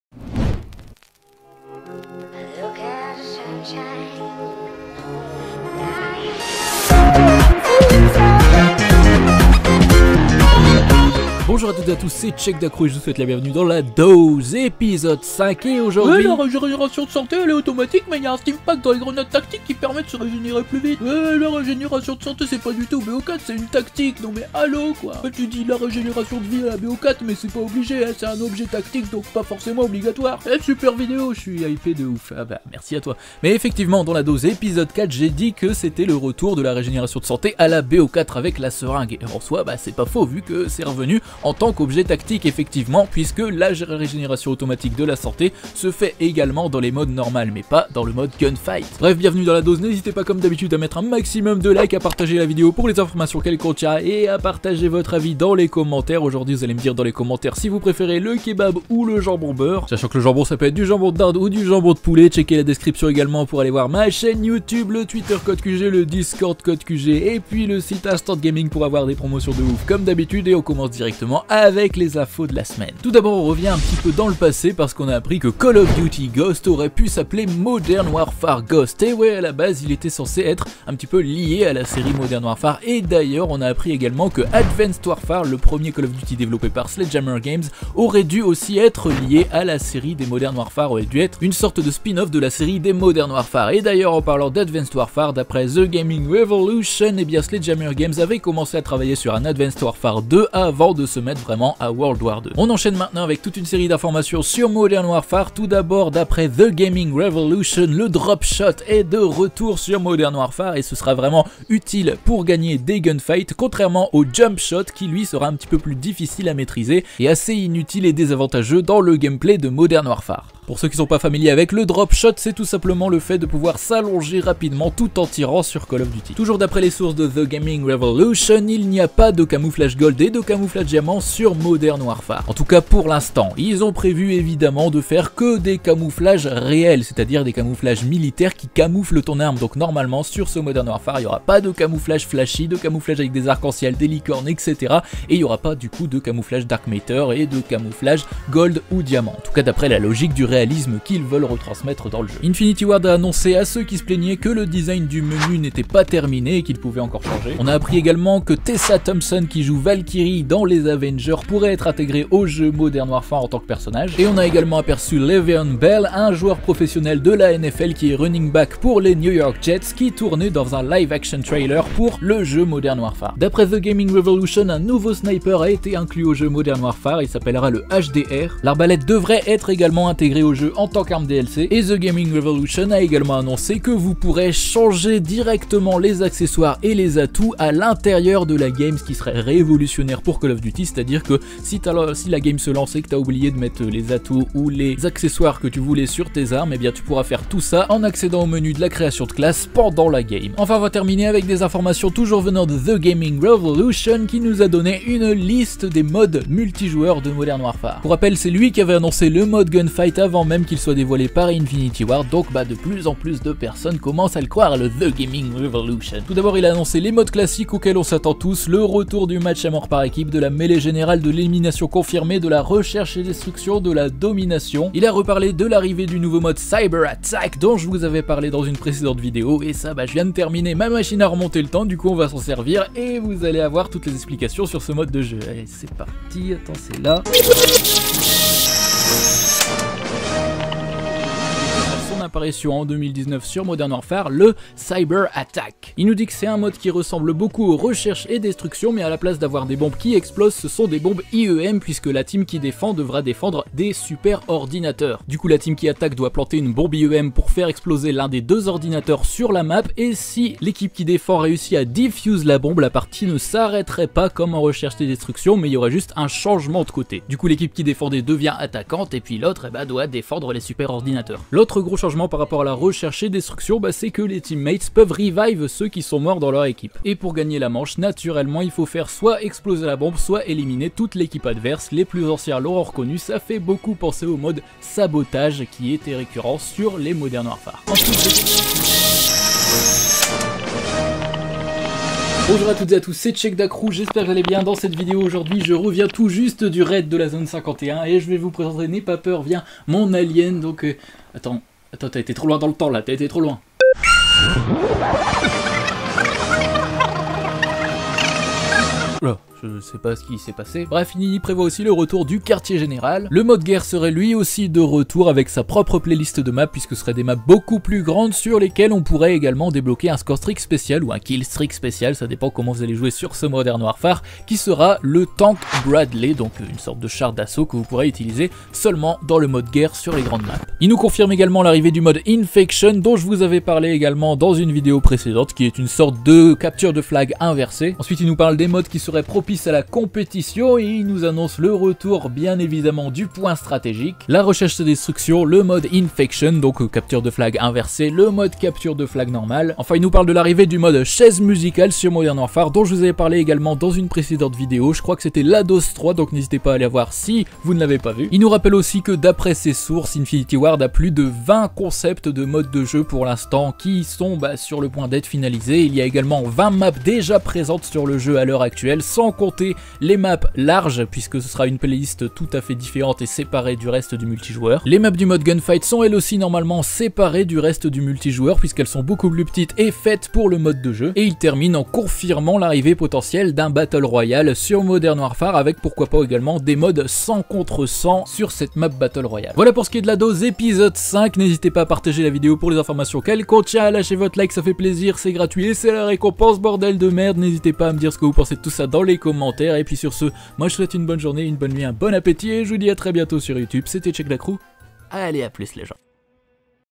MODERN WARFARE GHOSTS, MODE DE JEUX & ARMES (LA DOSE #5)
5ème épisode de "La Dose", votre série hebdomadaire regroupant toutes les petites infos sorties dans la semaine!
Retrouvez aujourd'hui le pleins d'infos sur Call of Duty: Modern Warfare avec des infos concernant les noms originels des précédents Call of Duty, les modes de jeux de COD: Modern Warfare, les armes, des acteurs, etc.